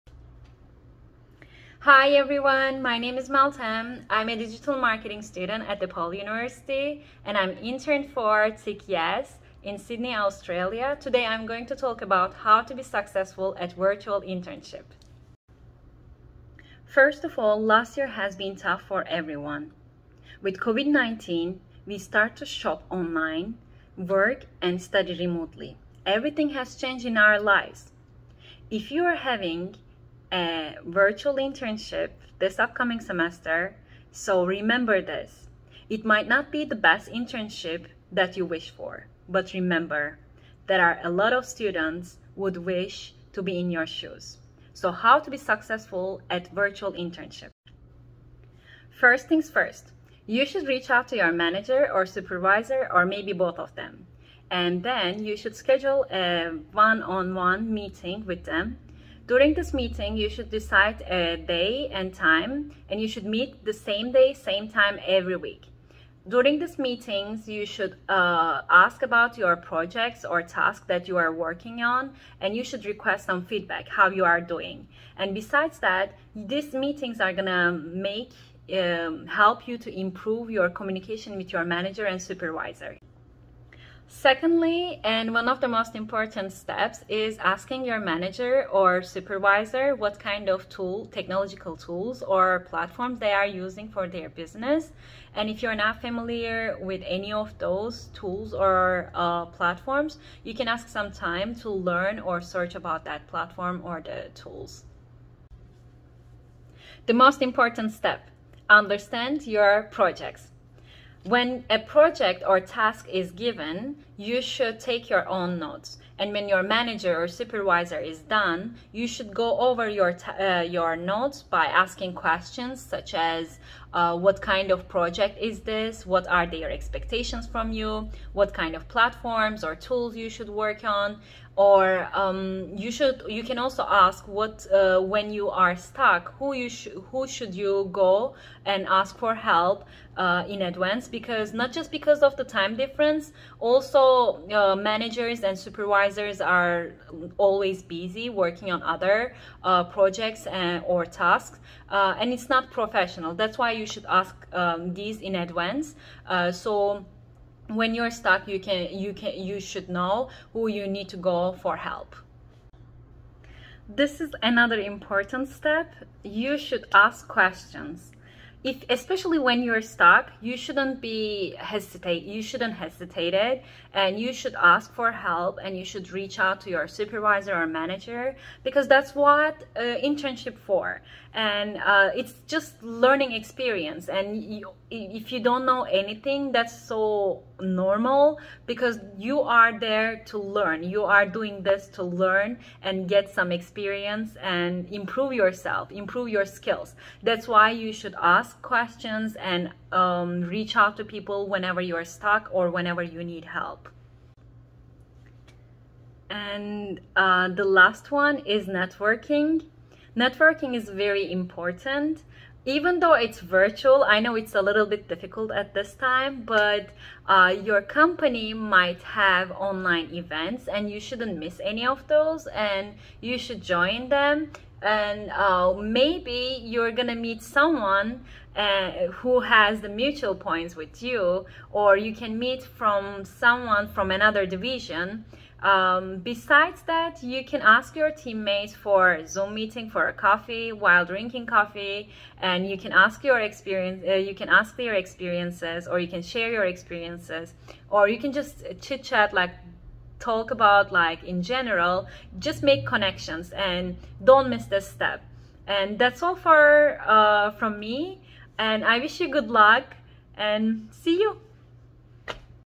My Virtual Global Internship: Sydney, Australia // CEA Study Abroad
COVID-19 has changed things completely for those who are looking for internships. We have many questions in our minds, like how to find an internship, how to reach out to the recruiters, or how to make certain decisions. Finally, you made it! You have an internship. Now what? This video will lead you to how to be successful at a virtual, international internship. Good luck!

(Meltem Sari, CEA MOJO Videographer - Summer 2021 - DePaul University)